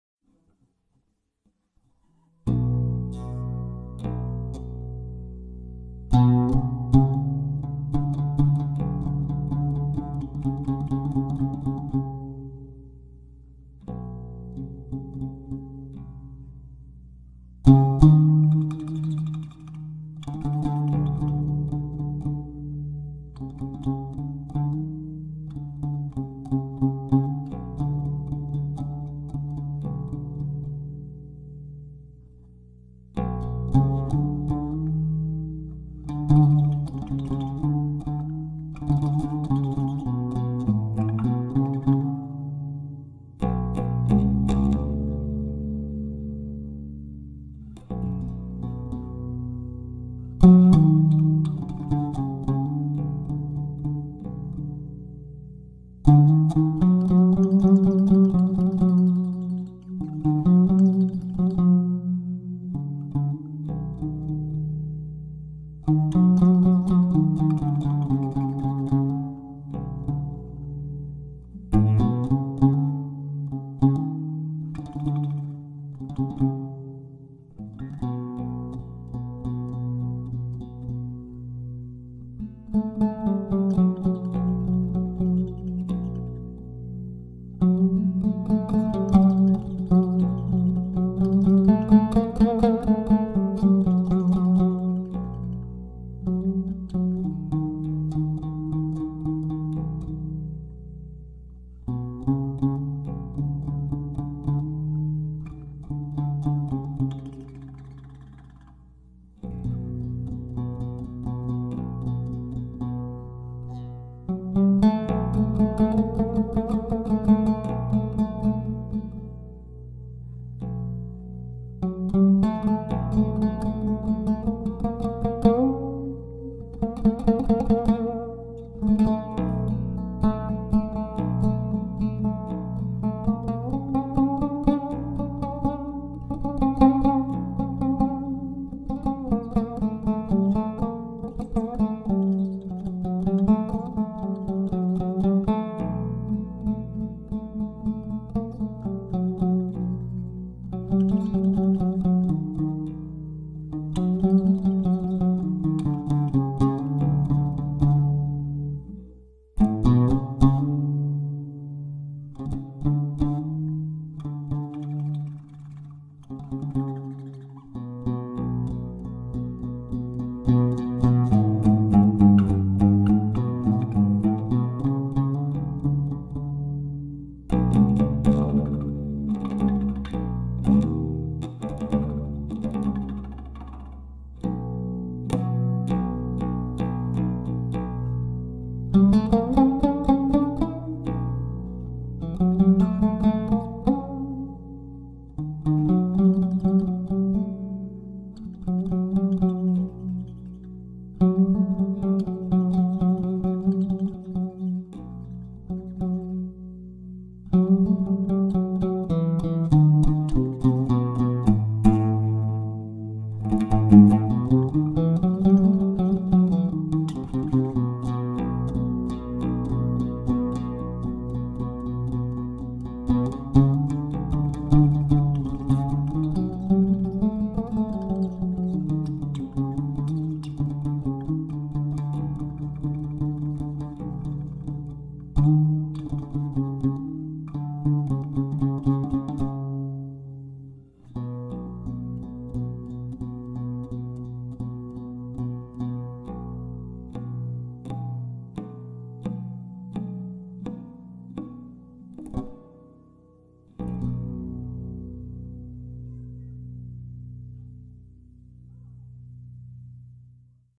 Saba OUD Taksim-Marco Sollazzo
©️2021 Marco Sollazzo, Saba Oud Taksim

Hello my name is Marco, I am Italian and I love playing the oud